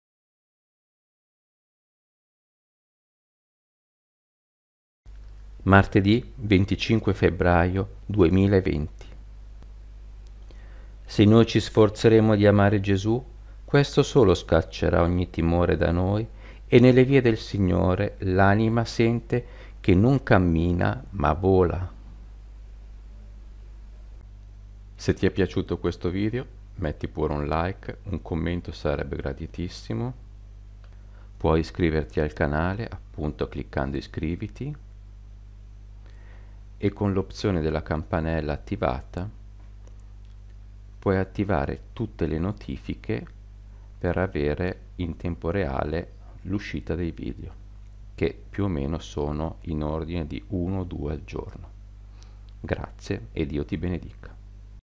Dal pensiero di San Pio del 25 febbraio.
Un pensiero per ogni giorno dell'anno con San Pio, è una raccolta
di frasi raccolte da un libro della shalom in cui ogni giorno vi è un suo pensiero.
a memoria di questo santo che tanto ha fatto per la chiesa cattolica e per tutti noi.

La mia pagina facebook è: Raniero Martinelli.